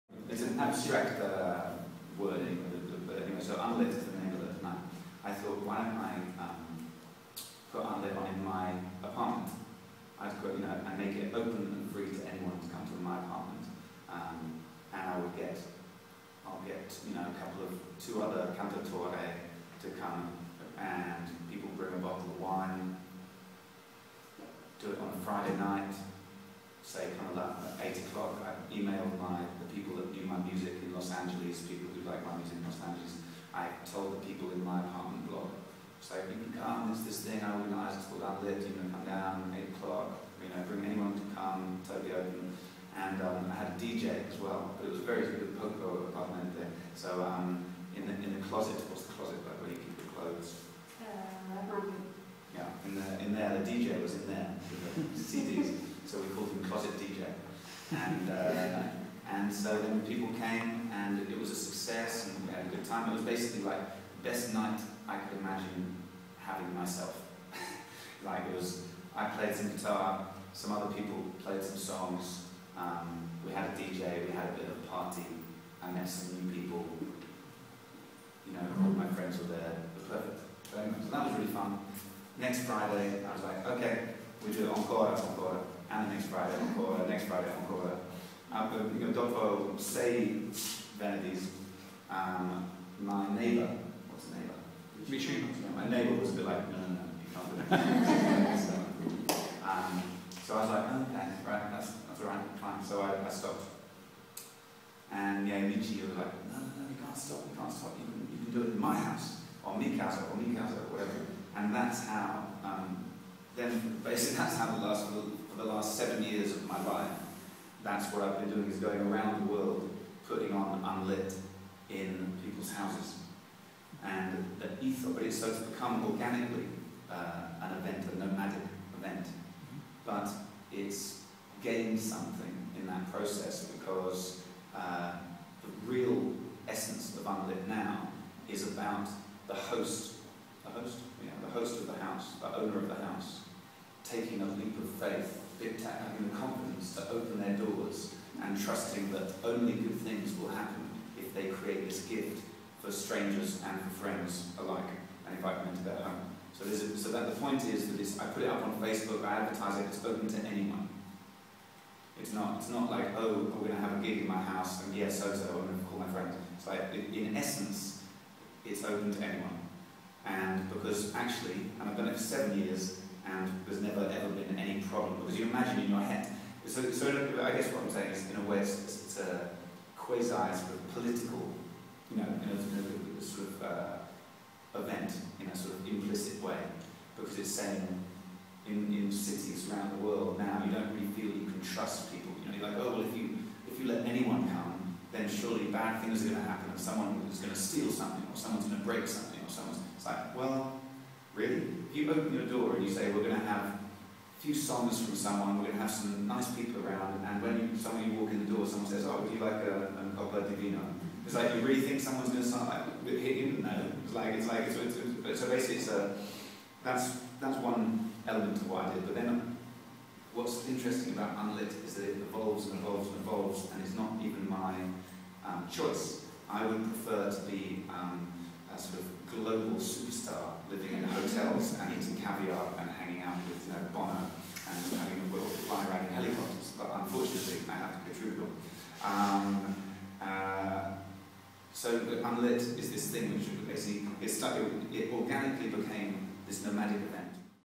eva meets Jont part 1/7
Jont tells about his UNLIT project.